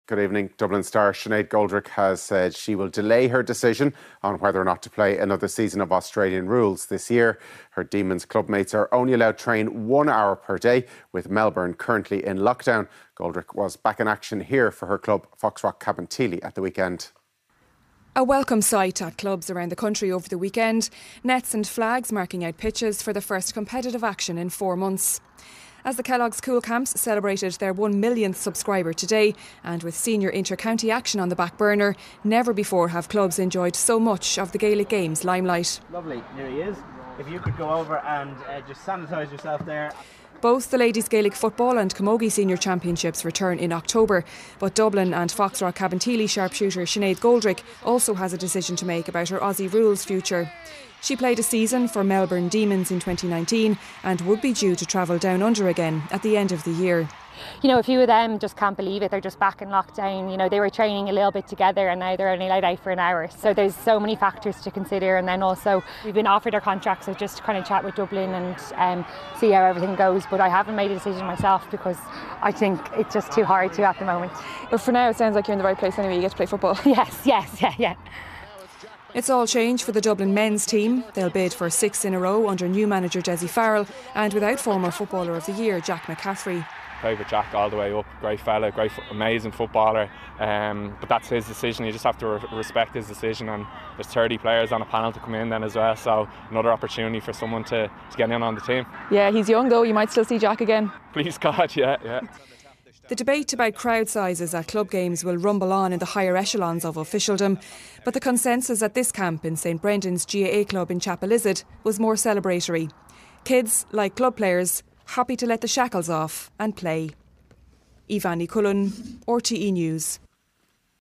Sinéad Goldrick
Dublin star Sinéad Goldrick is unsure whether she will be heading back to Australia despite being offered a second year with Melbourne in the AFLW.